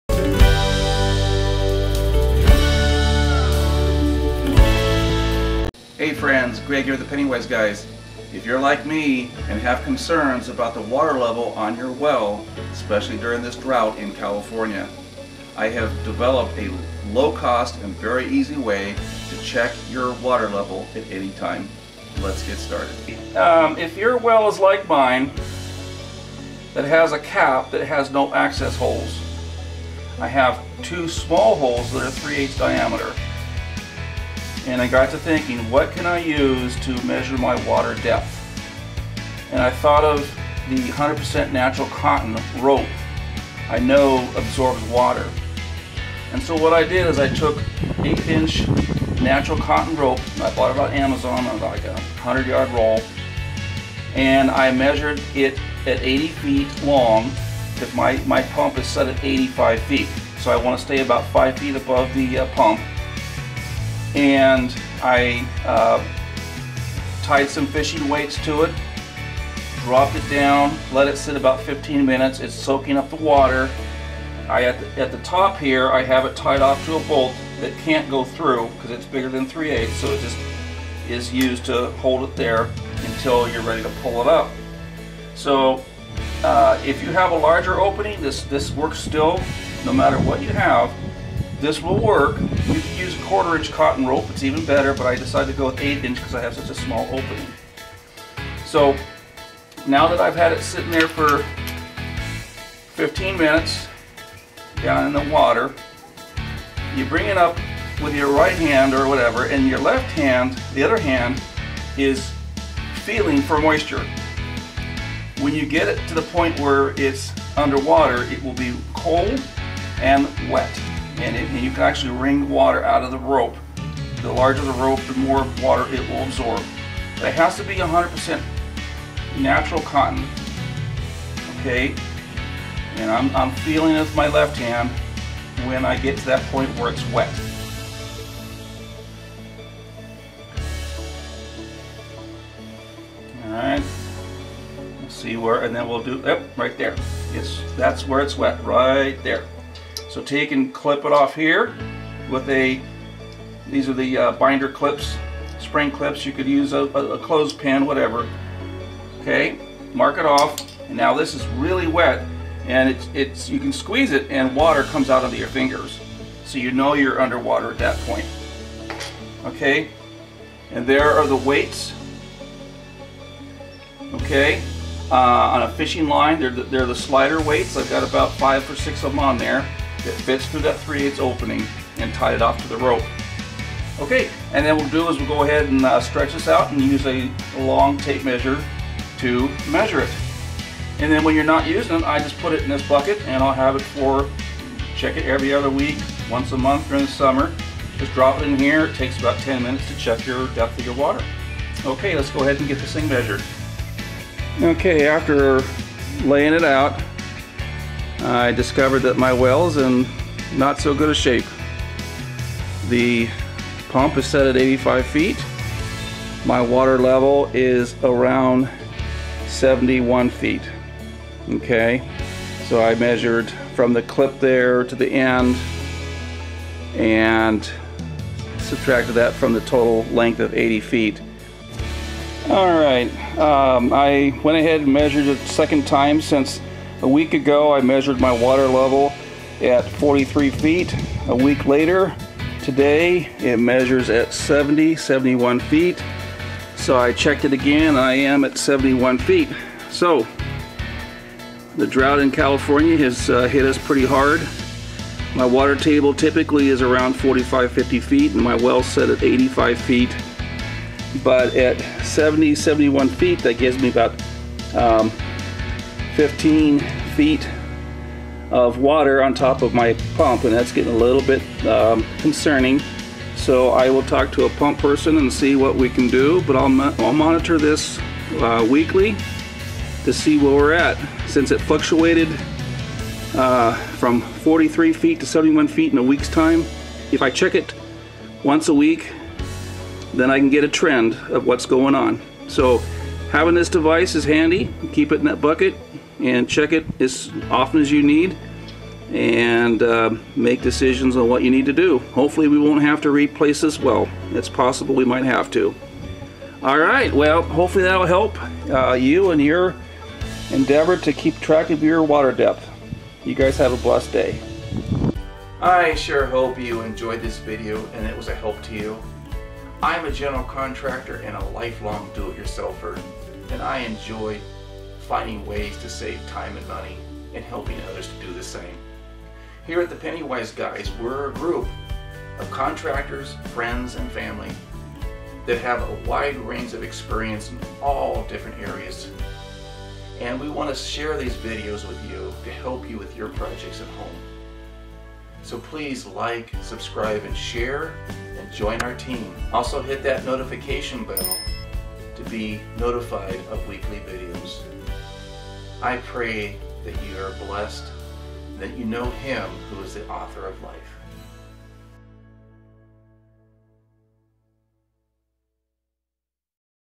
Checking Water Level on a Domestic Well when Access is less than ½” hole
How to check the water level on your domestic well that has an access hole that is less than 1/2 inch. This cheap and easy method takes only a few minutes and can be used to check daily to help evaluate water level trends.